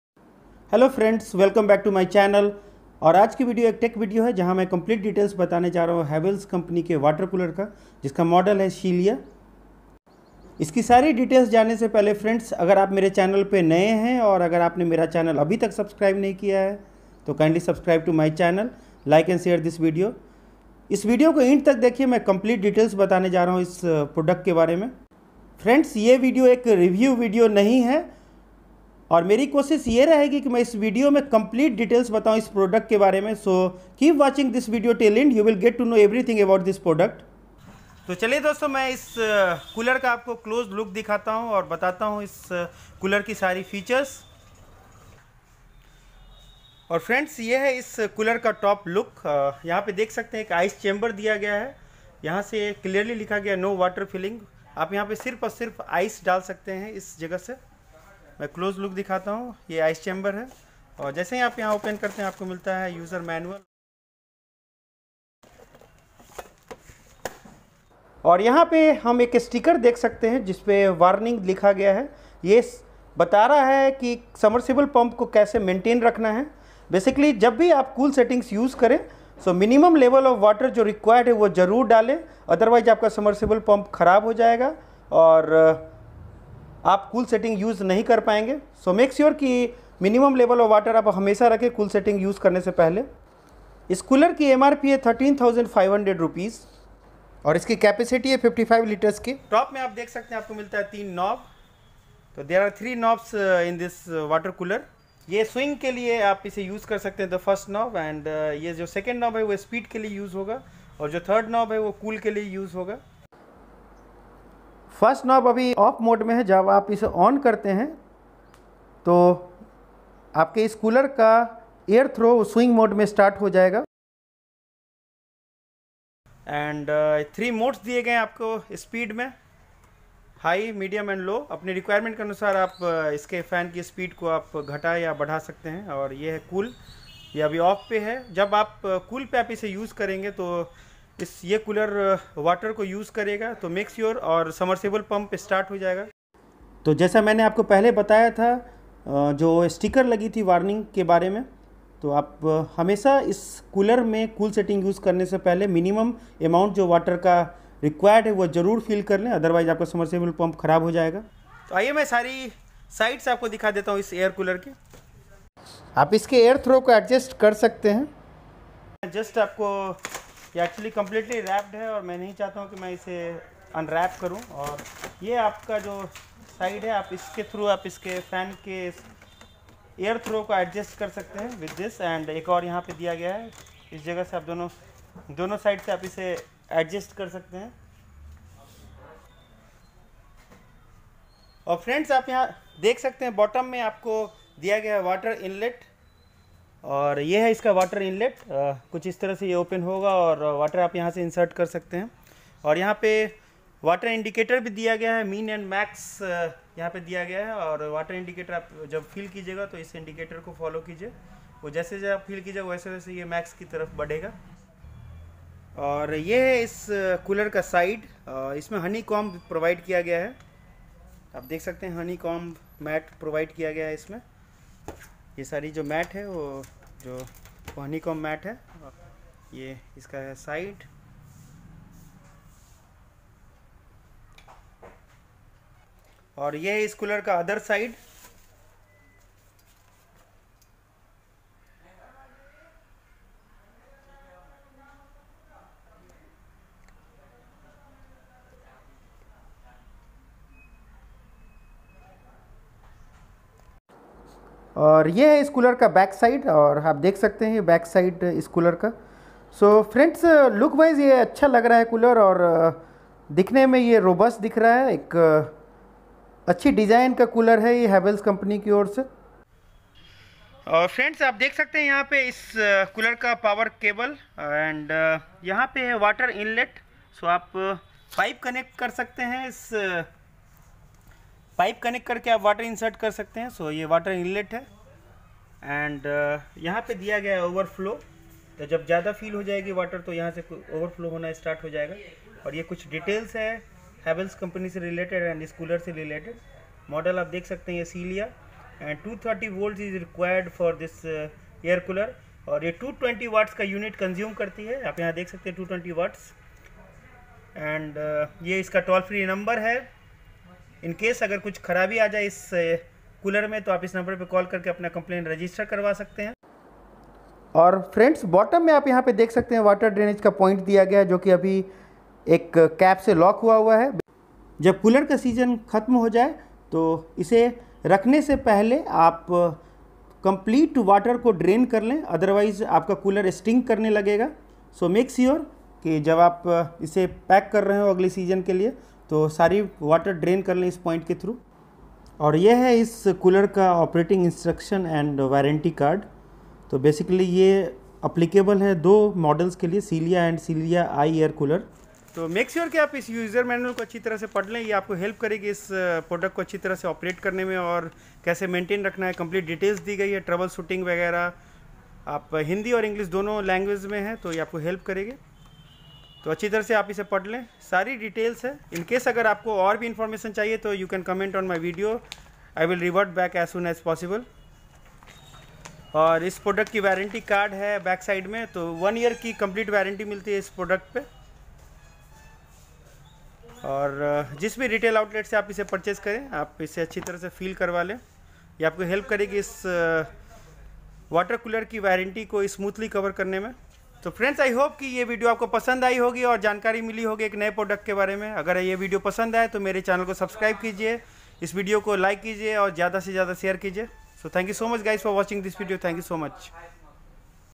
Havells 55L CELIA Desert Water Cooler Full Details
In this video, We have covered the complete information about HAVELLS Celia Air Cooler which includes:
   1. Feature Explanation of HAVELLS Celia Air Cooler
   2. First Impression of HAVELLS Celia Air Cooler

  Brand: HAVELLS
  Type: Air Cooler
  Model:CELIA
  Capacity: 55 Litres
  MRP:13500
  Power Consumption: 220Watts

Amazon Link for more Cooler Options:

## Aapko agar koi detailed video chhaiye kisi electronic/electric products pe, aap comment karein, I will try my best to upload video on the product.